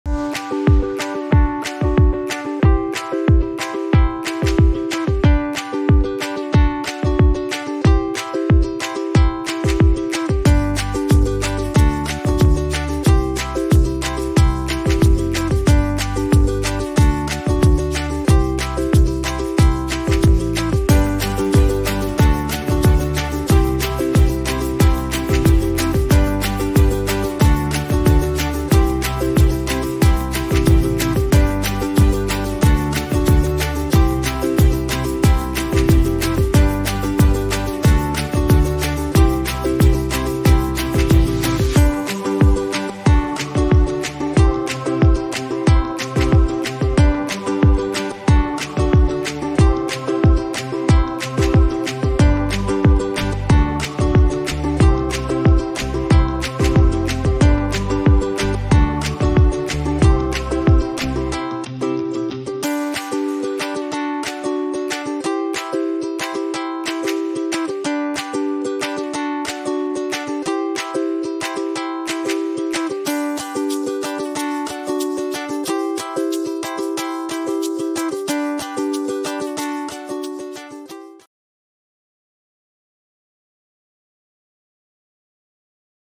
Kid's Health: You can help your child's memory by giving them these foods | The Health Site
Kid's Health: These days, kids typically have memory problems. They occasionally forget things, which leads to subpar academic performance. Feed your children these meals to help them achieve good grades if you want them to have a keen memory.